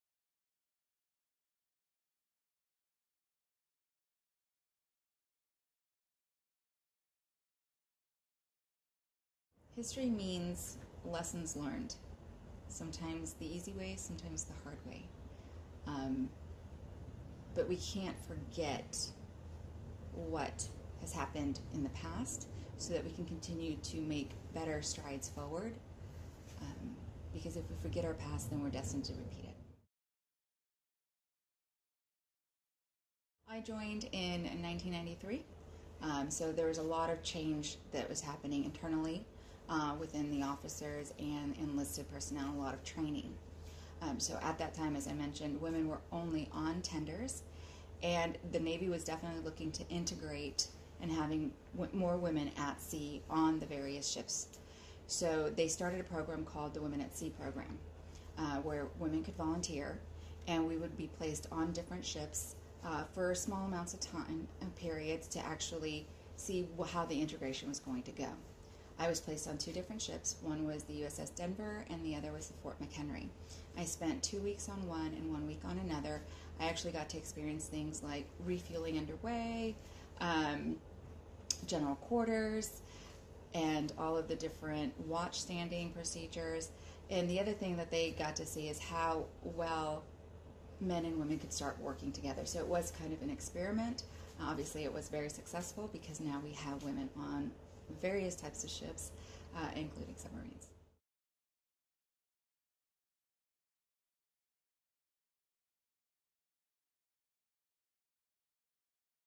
Information Systems Technician 1st Class Dora-Lee Arjona, mobilized to Camp Lemonnier, Djibouti
DJIBOUTI, Djibouti (Mar. 10, 2022) Information Systems Technician 1st Class Dora-Lee Arjona, mobilized to Camp Lemonnier, Djibouti, Africa, recalls her experience in 1993, at the start of her Navy career, with the Women At Sea program. (U.S. Navy video by Mass Communication Specialist 1st Class Christopher Previc)

Women At Sea
DJIBOUTI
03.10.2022

Film Credits: Video by Petty Officer 1st Class Christopher Previc
Camp Lemonnier, Djibouti